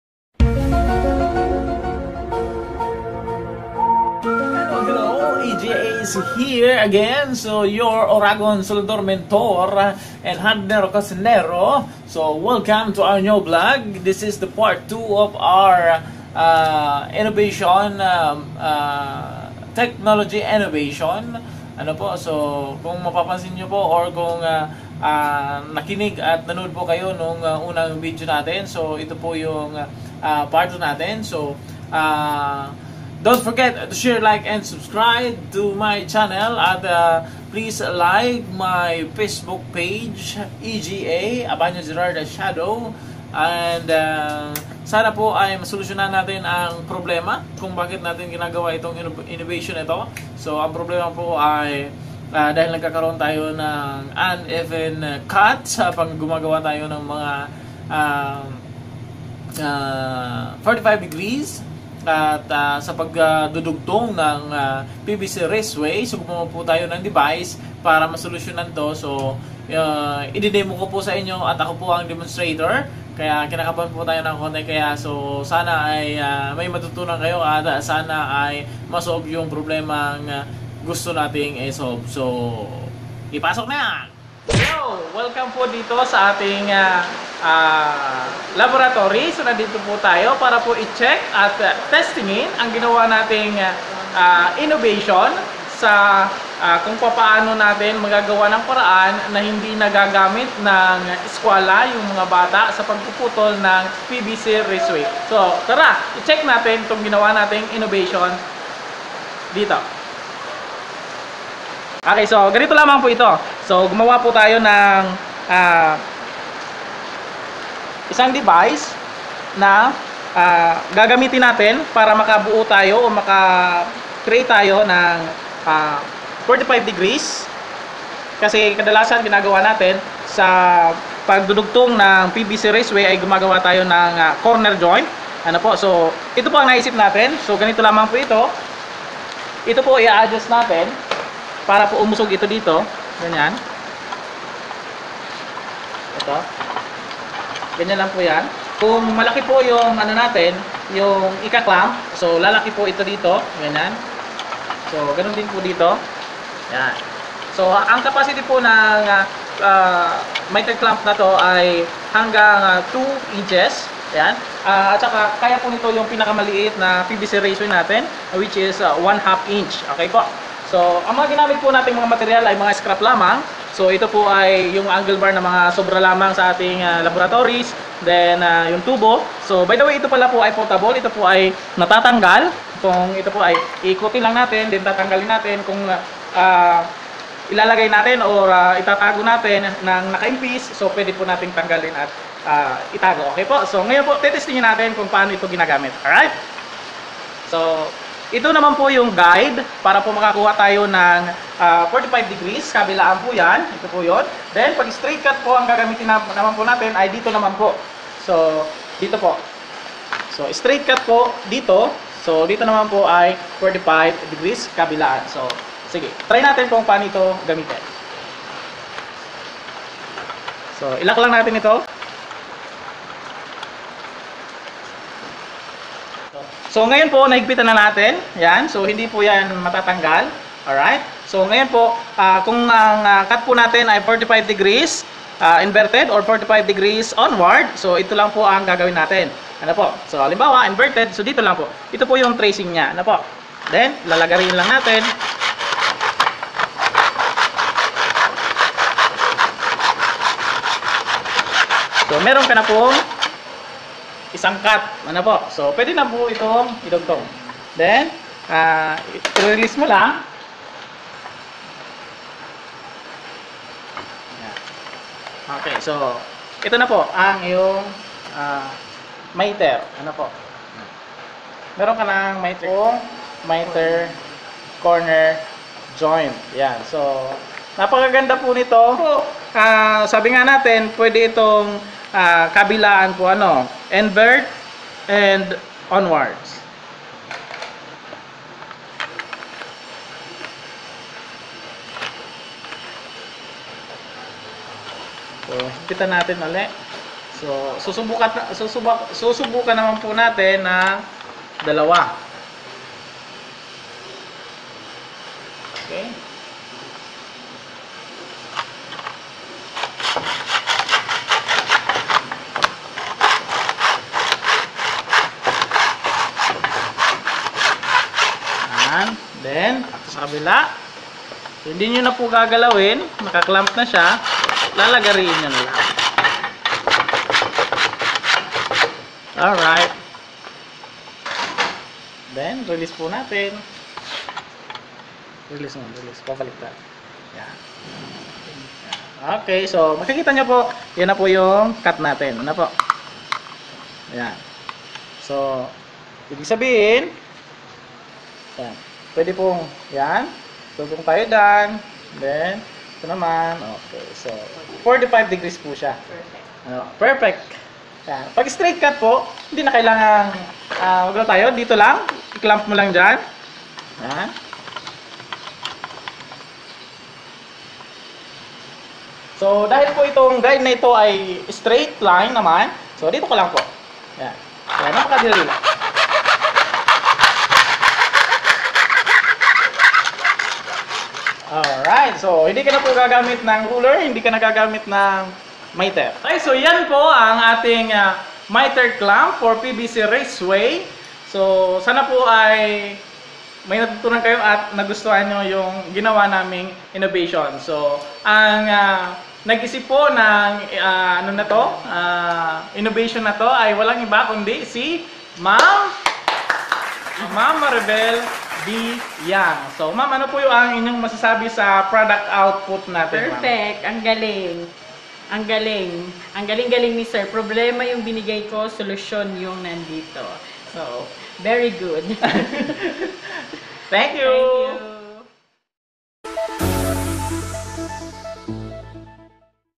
D.I.Y. 45degrees MITER CLAMP for PVC RACEWAY part 2
Naimbitahan Po Tayo na magdesinyo Ng isang device na maaring makatulong sa CSS course upang mapadali Ang pag puputol at pag cut nila Ng 45degrees sa PVC raceway
The problem;
Mahirap Ang pag cut Ng pvc raceway na naka 45degrees
Madumi sa lamisa at nasisira dahil sa natatamaan Ng lagari Ito.
Solosyon;
PVC 45DEGREES MITER CLAMP PVC RACEWAY
- D.I.Y miter clamp part 1
– D.I.Y. miter clamp part 2